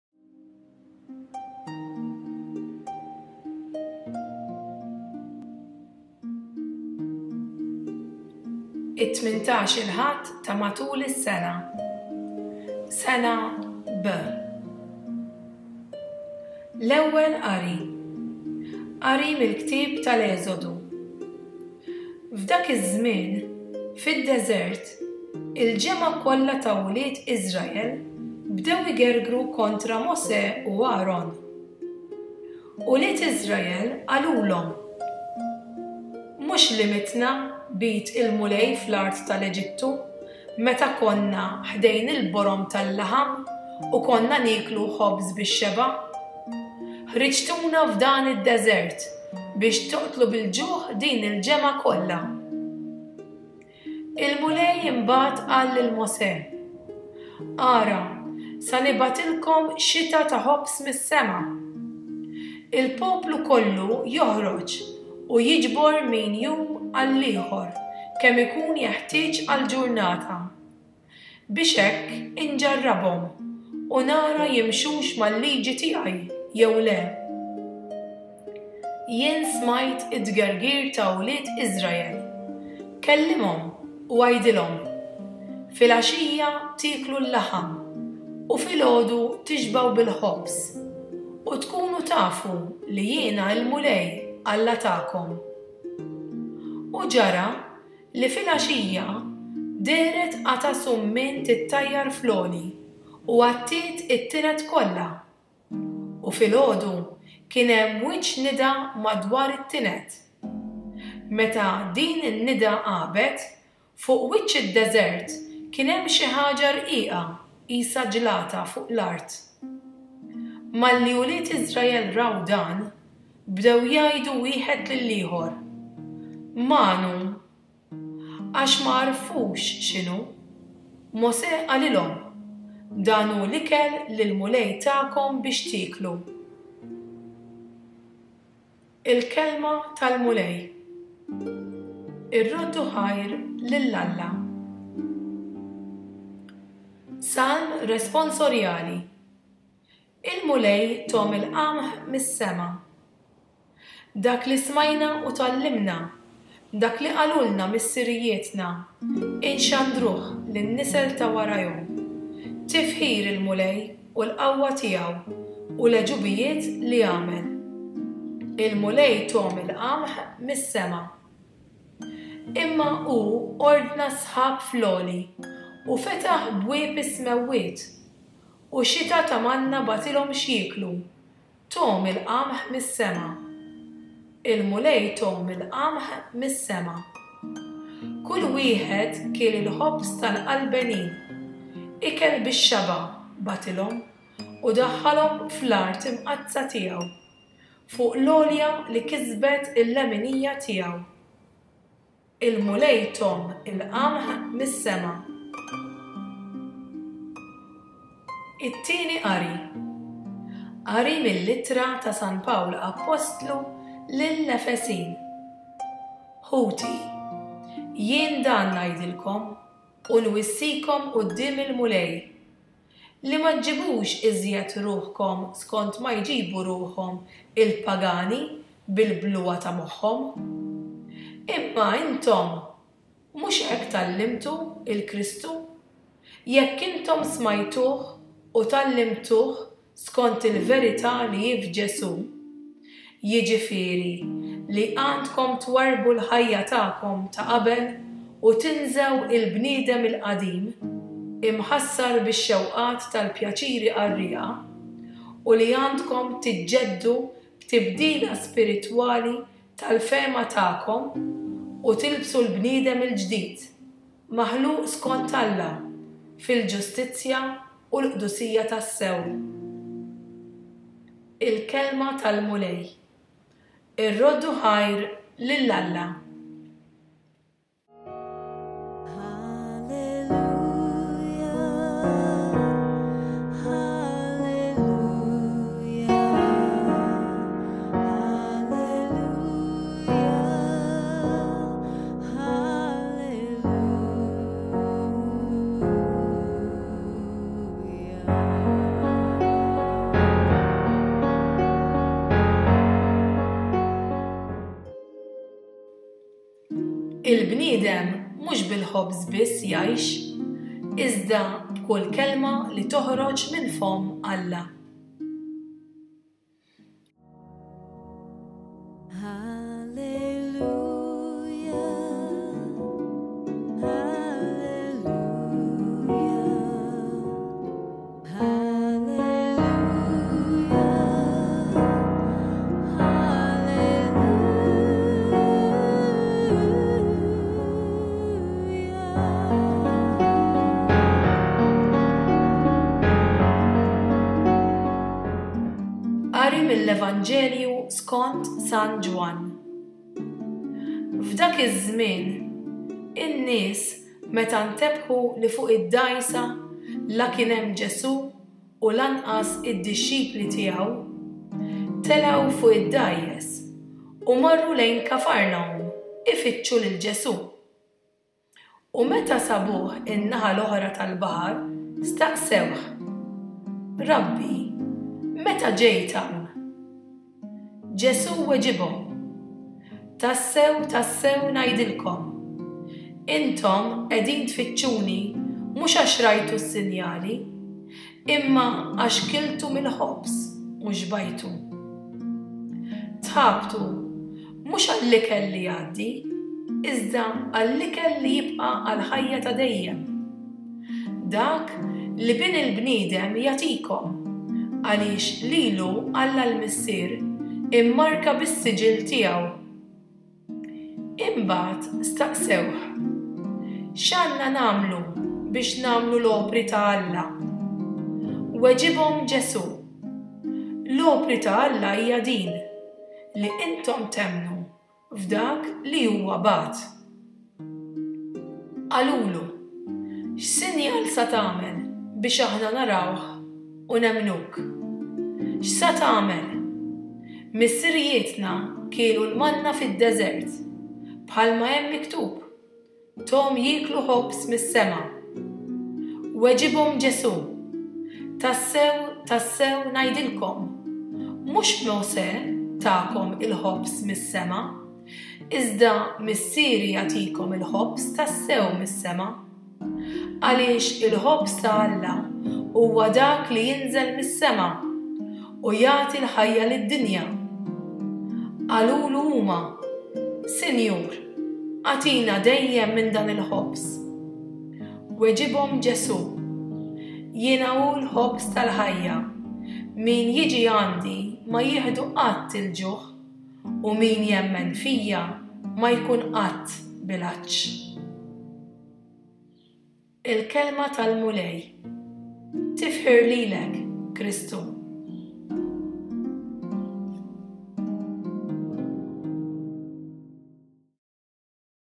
It-18 -il Ħadd ta' Matul is-sena. Sena B
Qari tal-Liturġija tat-18 -il Ħadd ta' matul is-sena. Sena B